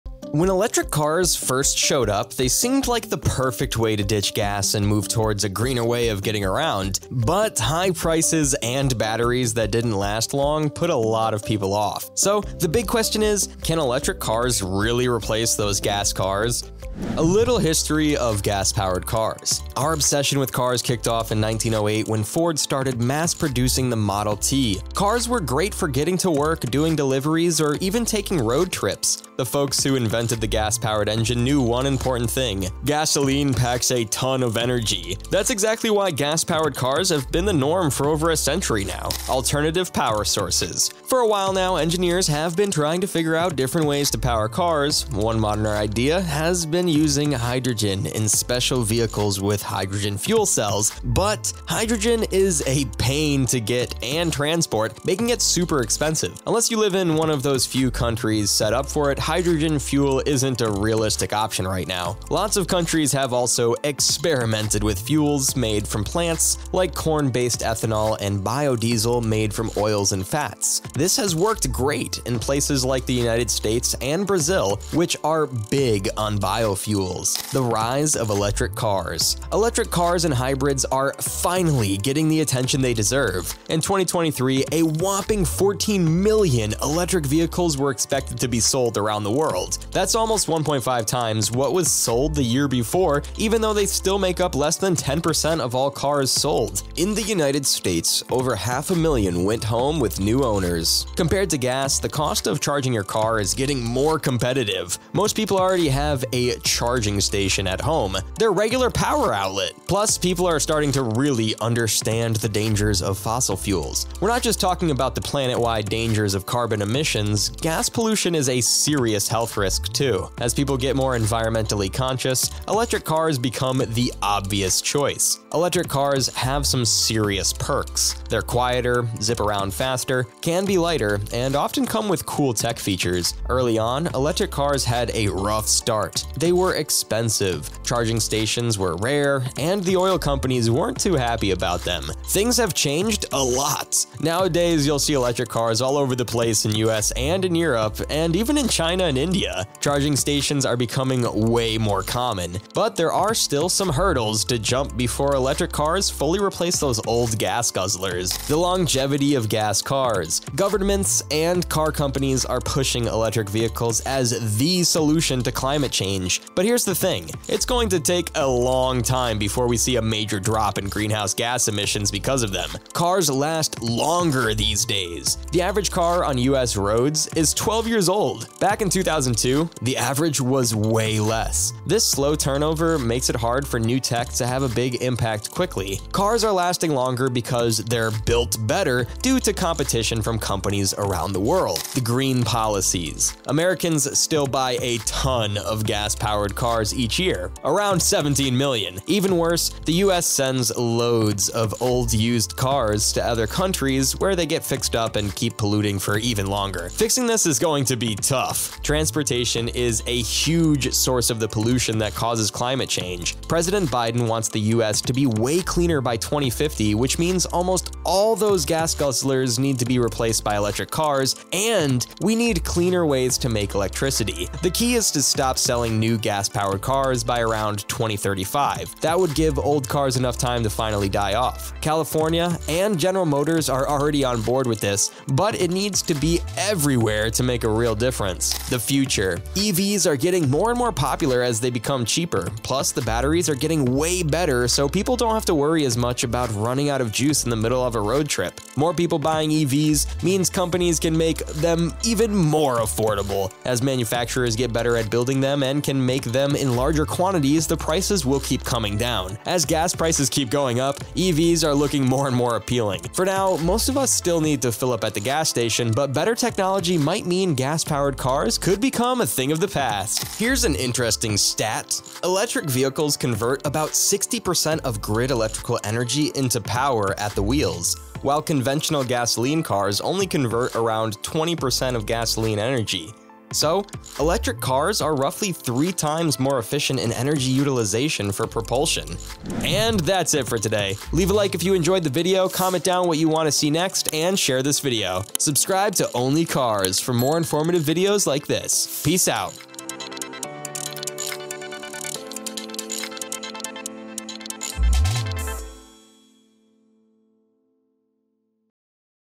Can Electric Vechile REALLY Replace Gas Powered Cars?
Electric Vehicles (EVs) are surging! But can they fully replace gas-powered cars? This video explores the rise of EVs, dives into charging costs, range anxiety, and environmental impact. Plus, we look at how long gas cars might last & the challenges of going electric. Is the future electric? Watch now to find out & see what it means for YOU!

00:00 Intro
00:17 A Little History of Gas-Powered Cars
00:39 Alternative Power Sources
01:17 The Rise of Electric Cars
02:38 The Longevity of Gas Cars
03:11 The Green Policies
03:58 The Future

Check the HighStrideUk shop if you are interested.
Tailored Excellence: Premium Men's Trousers